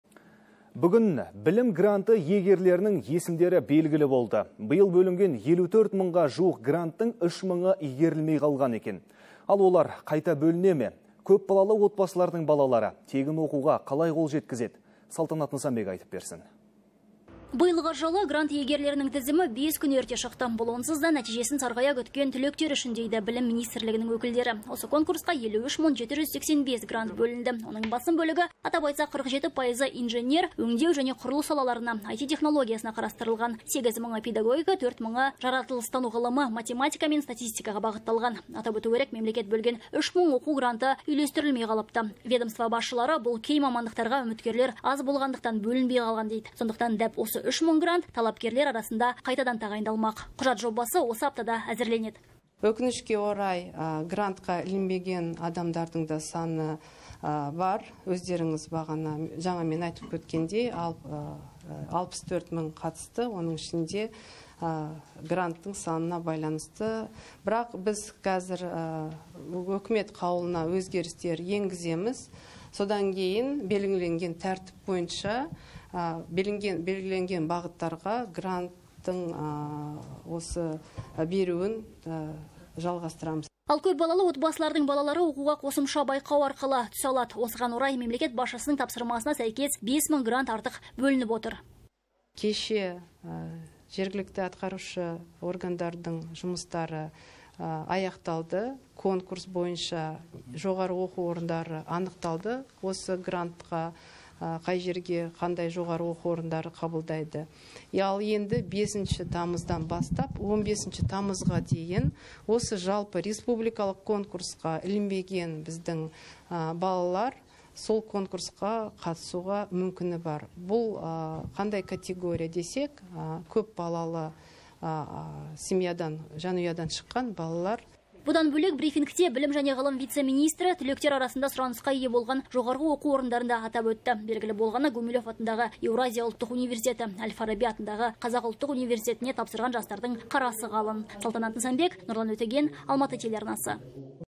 Бүгін білім гранты иегерлерінің есімдері белгілі болды (01.08.19)
Биыл грант иегерлерінің тізімі бес күн ерте шықты. Бұл онсыз да нәтижесін сарғая күткен түлектер үшін дейді білім министрлігінің өкілдері, деп хабарлайды Almaty.tv тілшісі.